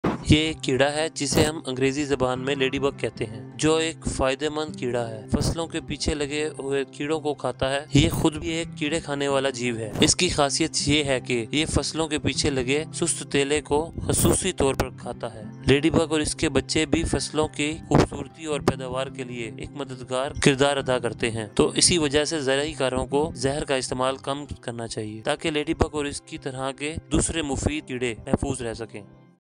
The Secret Life of Ladybugs
In this short video, we delve into the fascinating world of ladybugs, known as "Ladybug. These tiny creatures play a crucial role in maintaining ecological balance by preying on pests that harm crops. Despite their small size, ladybugs are mighty warriors against garden nuisances. Discover how they survive by feeding on pests found on the back of leaves, showcasing their special talent for hunting. Let's explore why reducing the use of pesticides is essential to protect not only ladybugs but also other beneficial insects. Join us in celebrating the beauty and usefulness of ladybugs in our ecosystem!